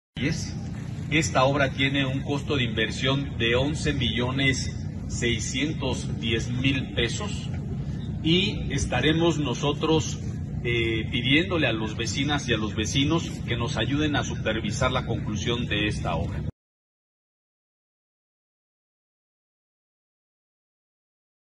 Eduardo Rivera supervisa la rehabilitación vial con concreto asfáltico del Bulevard Puebla.
El presidente municipal Eduardo Rivera Pérez supervisa la rehabilitación vial con concreto asfáltico del Bulevard Puebla.